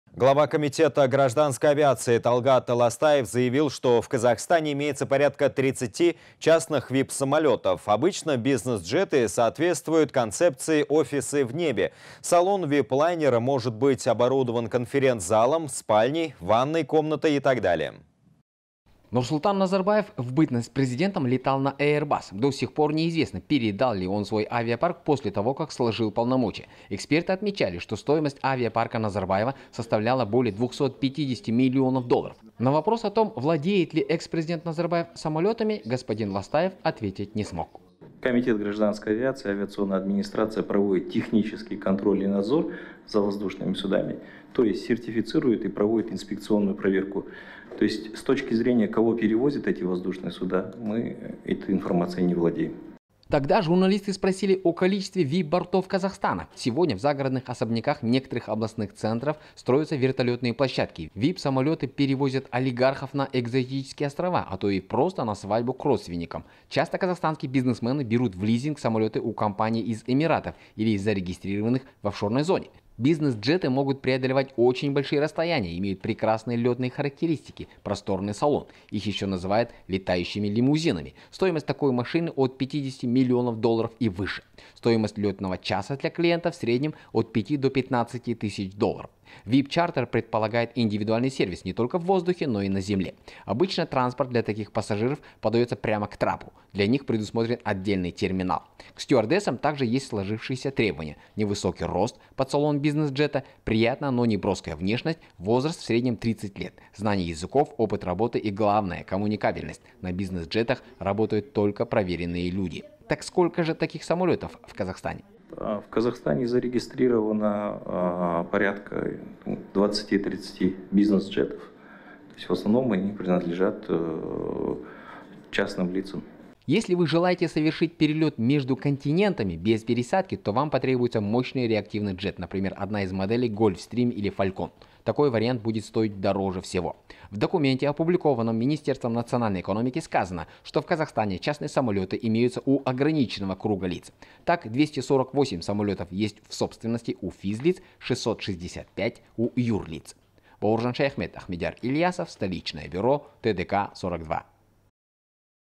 Сколько в Казахстане бизнес-джетов?
Нурсултан Назарбаев, в бытность президентом летал на Airbus. До сих пор неизвестно передал ли он свой авиапарк после того, как сложил полномочия президента. Эксперты отмечали, что стоимость авиапарка Назарбаева составляет более 250 млн долларов. На вопрос о том, владеет ли экс-президент Назарбаев самолетами Ластаев ответить не смог.